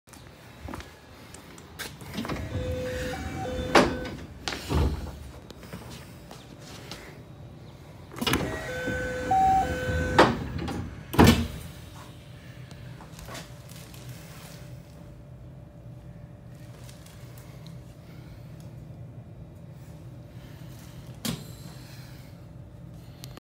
クモハ227ー95 半自動ドア操作
西条始発快速シティライナーに久しぶりに乗車したついでに半自動ドアを撮影